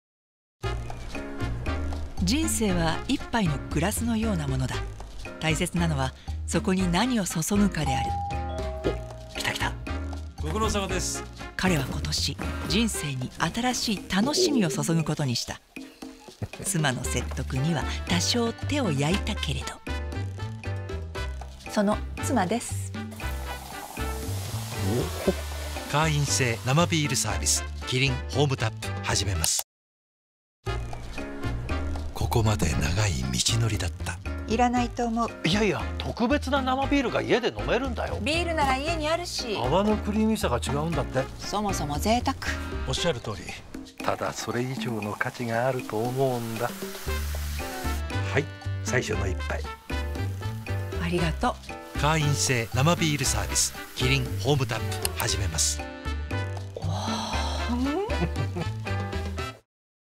【CM】天海祐希太太妥協讓丈夫中井貴一家中安裝生啤機只因疫情？
【CM】中井貴一天海祐希夫婦家中喝新鮮生啤！這對夫婦半年前為了是否安裝新機而爭執，太太說啤酒冰箱就有，但丈夫認為生啤完全不同，結果當然是太太話事，但半年後家中就安裝了，簡單安裝後就有新鮮的生啤。丈夫也懂得把第一杯獻給太太，問題就此解決。其實KIRIN的「Home Tap」服務2017年就開始但不太受關注，明顯是因為疫情關係大家無法外出喝生啤，所以KIRIN趁機會推出CM搶攻市場。你對家中喝生啤感興趣嗎？